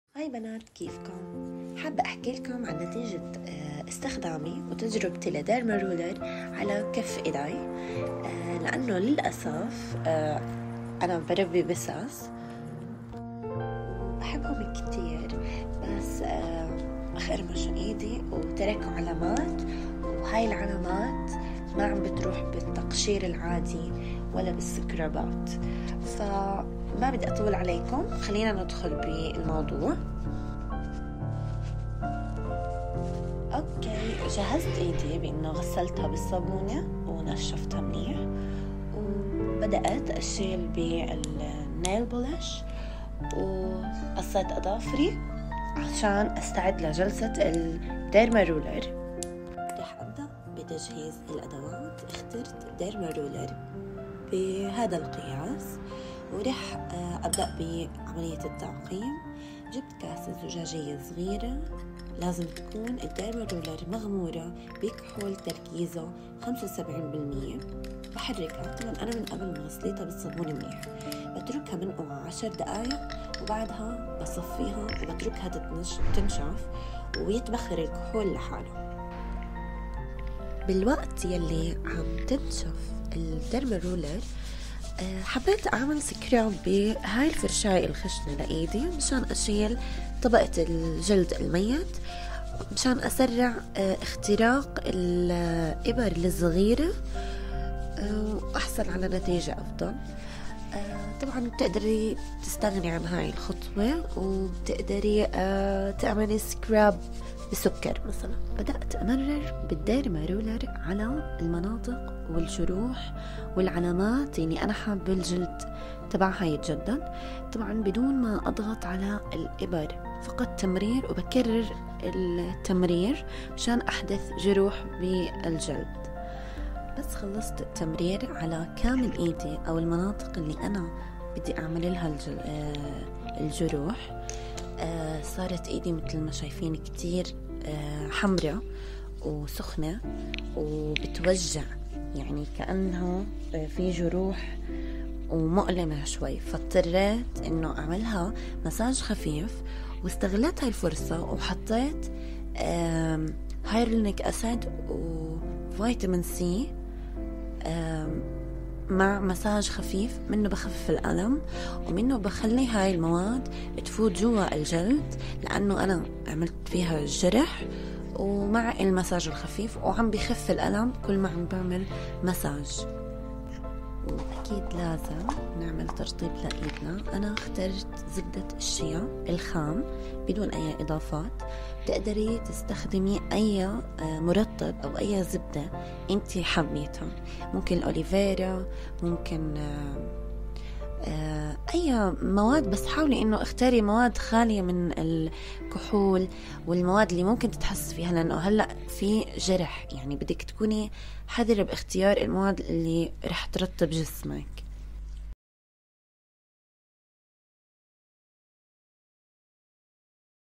بسهووولة وسرعة كيف اجعل يدي ناعمة بدون ندوب او تجاعيد باستخدام الديرما رولر
بناااات
والله جسمنا ربنا خلقه في احسن تقوييم الحمدلله والف الحمدلله
الديرما رولر مبداها رائع جدا بتحفز بشرتنا تنتج كولاجن وتعالج نفسها بنفسها